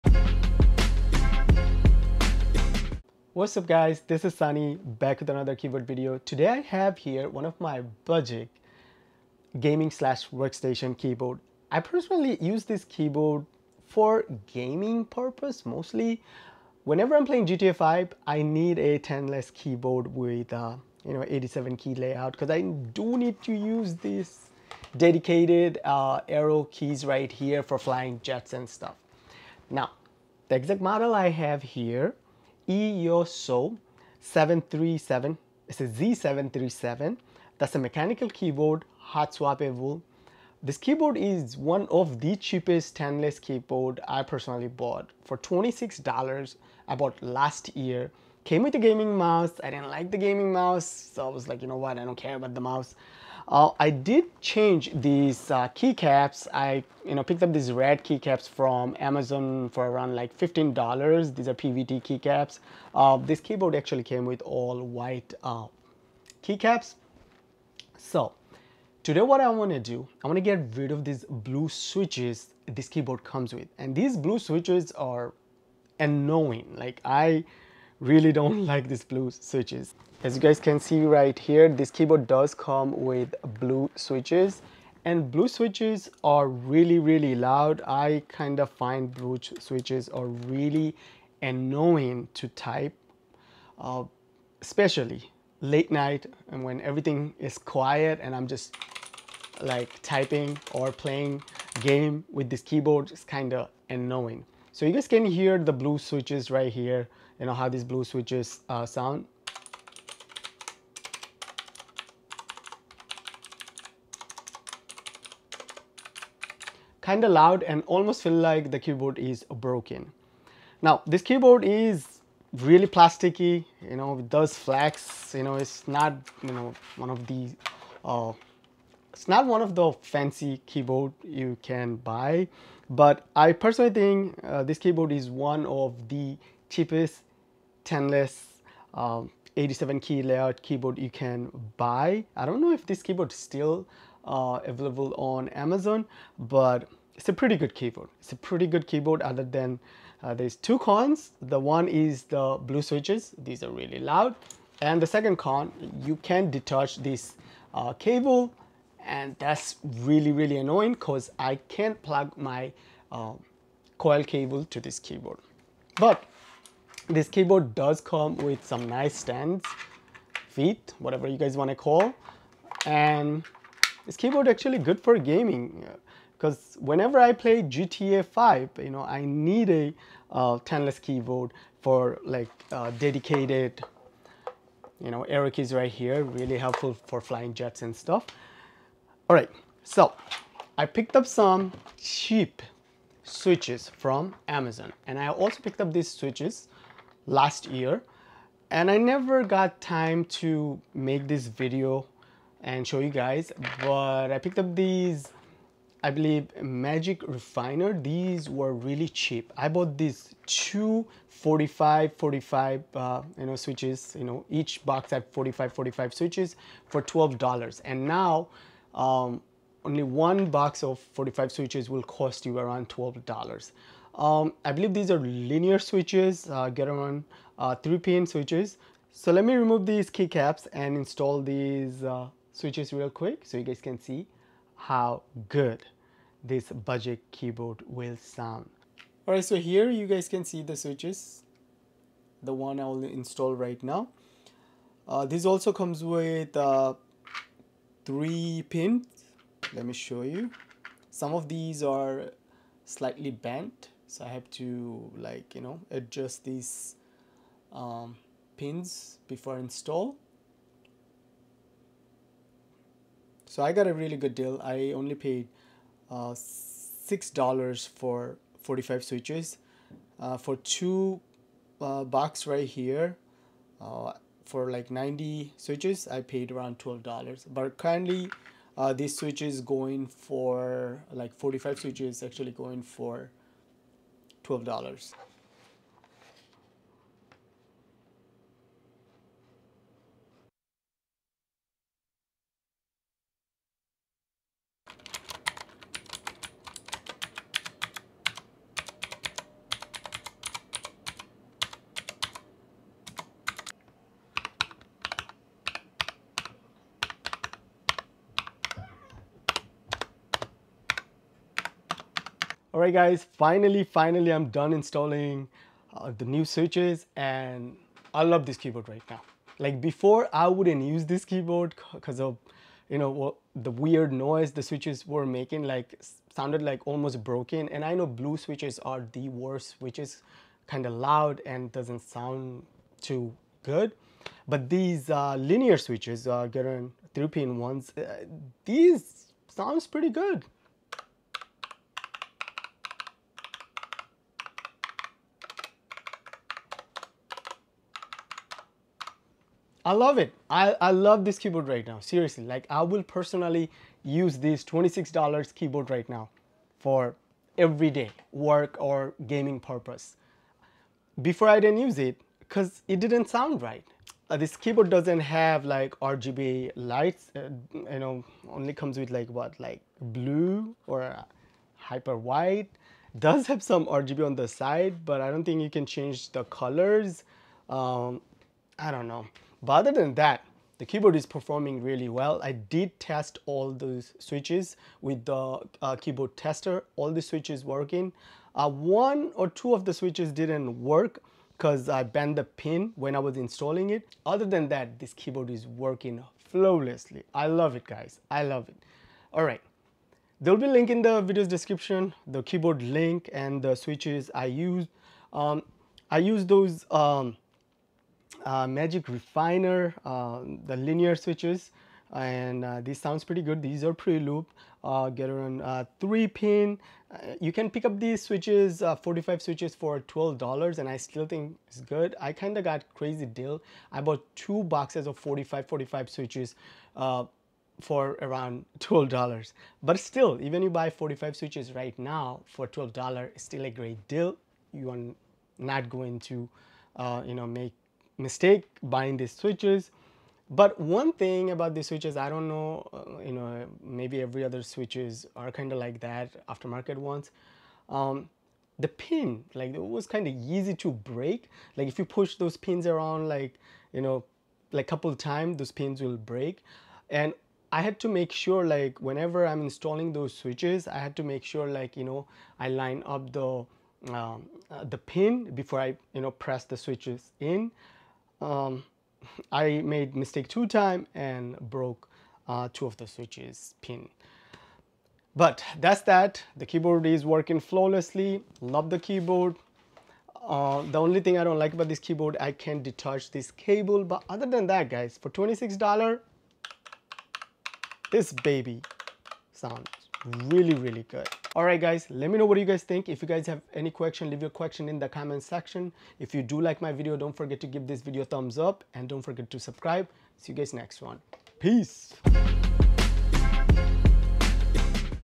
Budget Mechanical Hot Swappable Gaming Keyboard Switch Upgrade! (E-YOOSO Z737 K620)
I bought a cheap 87 keys mechanical Hot Swappable TKL gaming keyboard with blue switches. I decided to upgrade the switches.

E-YOOSO Keyboard
Jelly Yellow 45pcs Keyboard Switches

My Favorite 60% Mechanical Keyboards:
iLovBee i61
MageGee MK-Mini